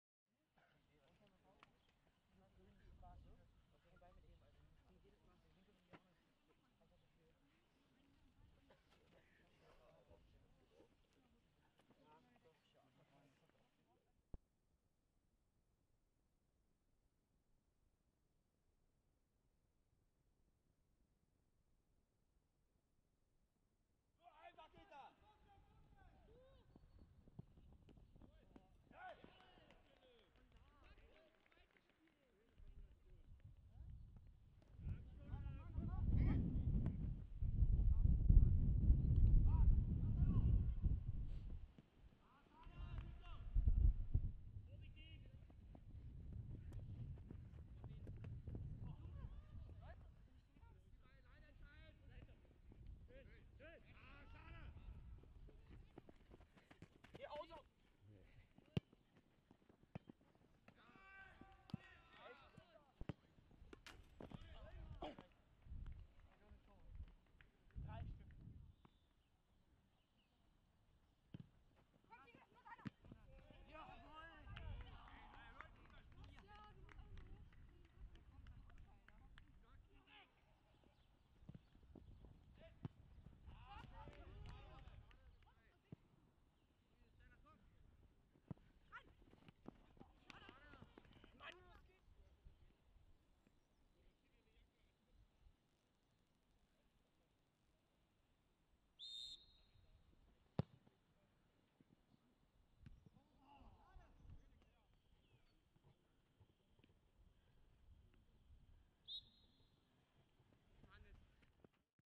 C-Junioren: SG Harz - TuS 1860 Magdeburg 1:4
C-Junioren Landesliga: SG Harz - TuS 1860 Magdeburg
Leider am Anfang etwas tonlos.